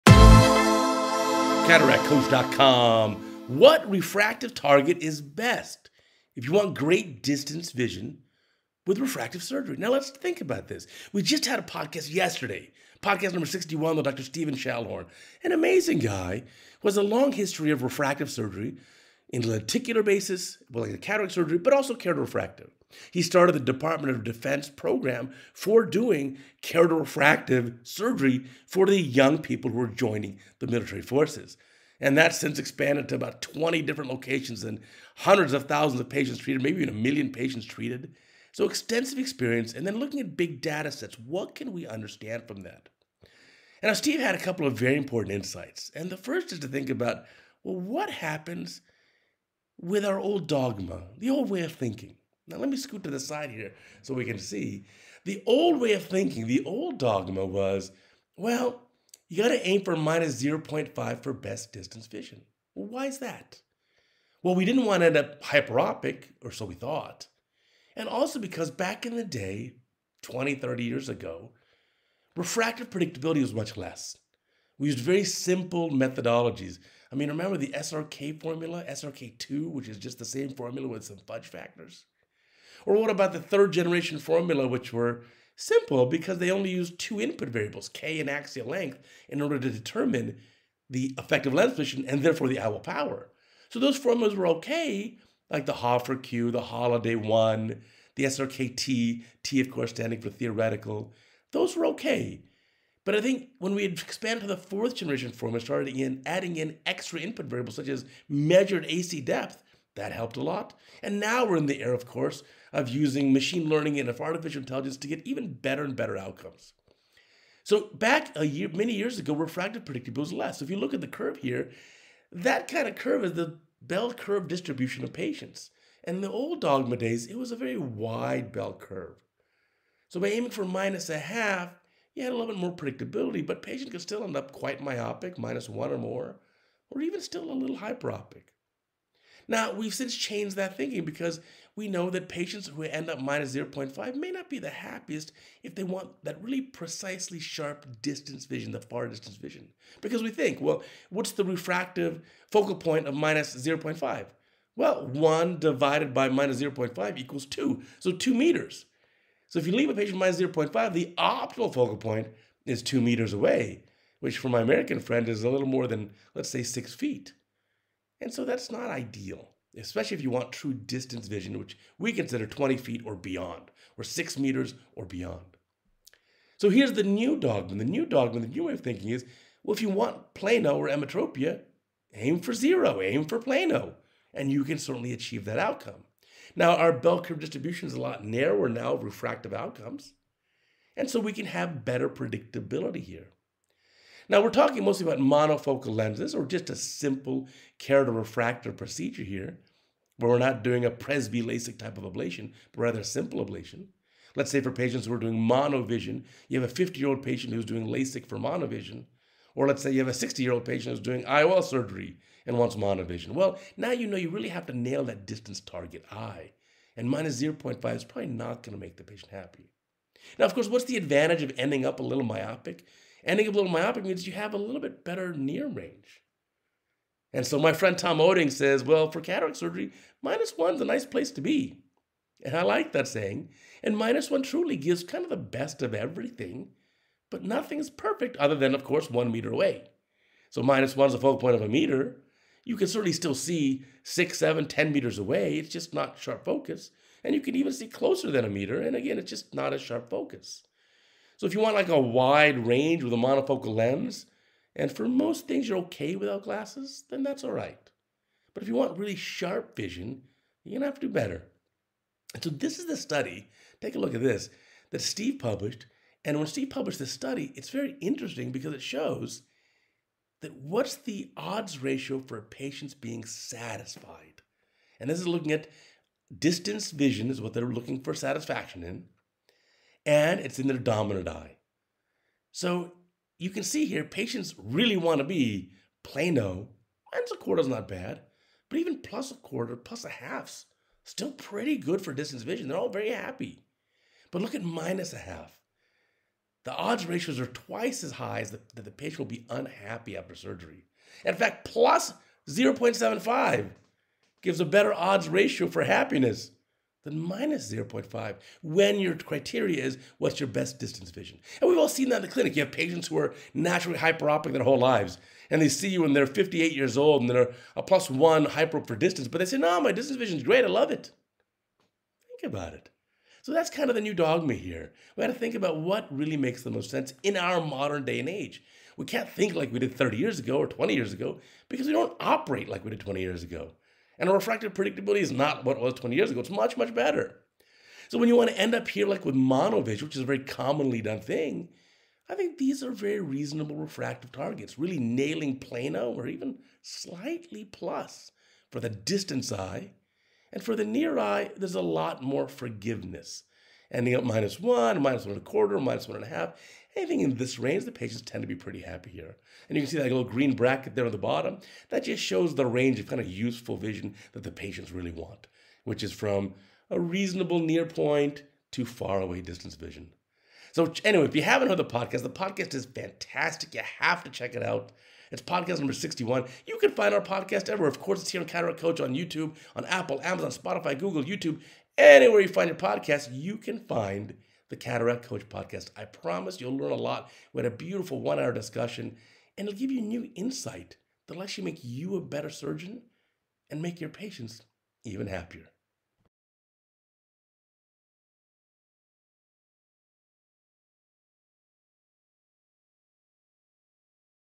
CataractCoach™ 2212: What refractive target is best for distance vision?
Yesterday's CataractCoach Podcast featured Dr Steven Schallhorn and it was a fascinating discussion that taught me so much. Using a big data approach he analyzed patient satisfaction after monofocal IOLs and a goal of plano for best distance vision. The results were surprising because they contradict the old dogma of cataract surgery that you should aim for -0.50 for best distance vision. That's not the best approach according to his data. Yup, there is even more benefit from listening to an hour podcast once a week! I am confident that you will love the podcast and you will learn so much to benefit your patients.